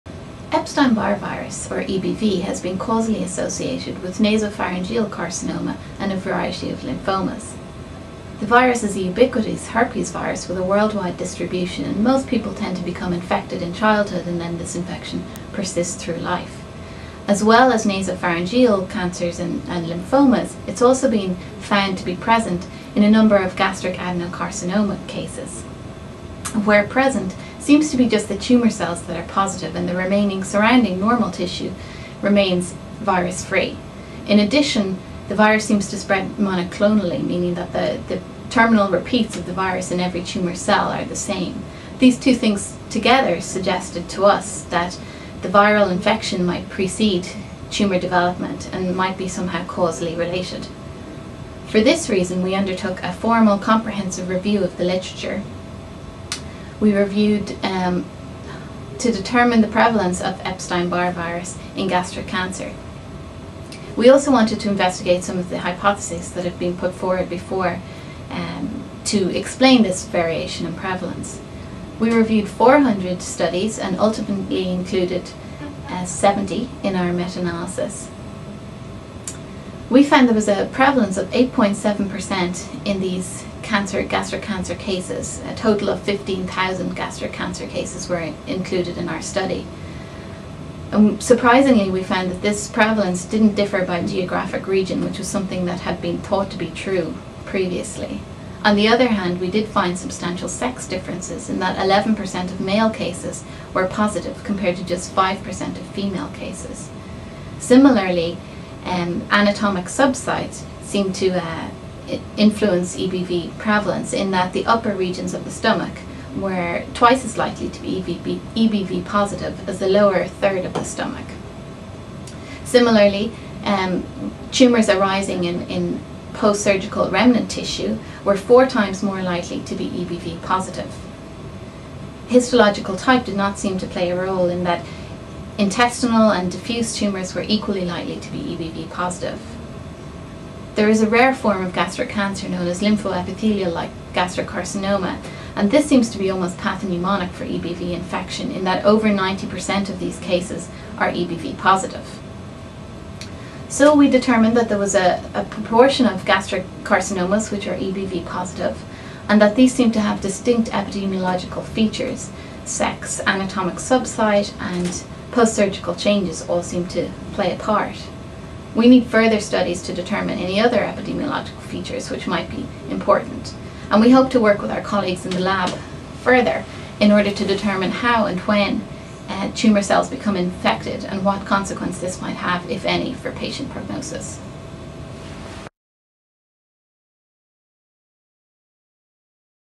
Meta-analysis Shows That Prevalence of EpsteinBarr Virus-Positive Gastric Cancer Differs Based...
Dr. Gwen Murphy discusses her manuscript Meta-analysis Shows That Prevalence of EpsteinBarr Virus-Positive Gastric Cancer Differs Based on Sex and Anatomic Location. To view the print version of this abstract go